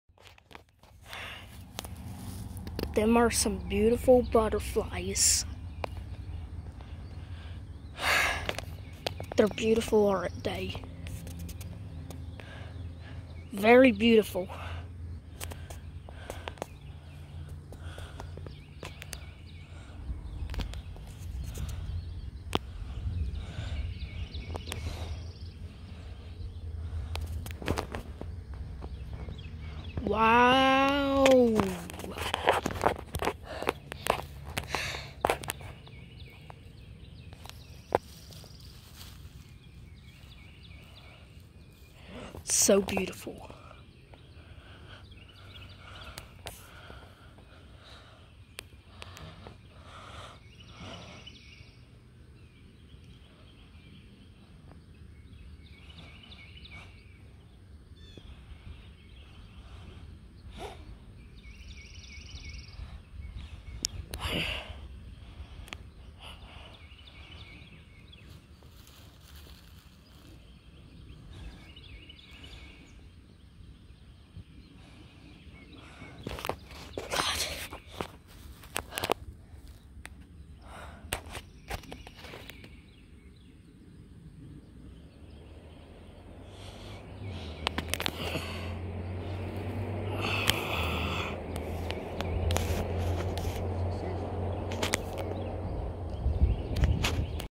Mac took this butterfly video of them feasting...
My boy is a budding content creator, learning how to use his camera. I'm proud of him for his efforts.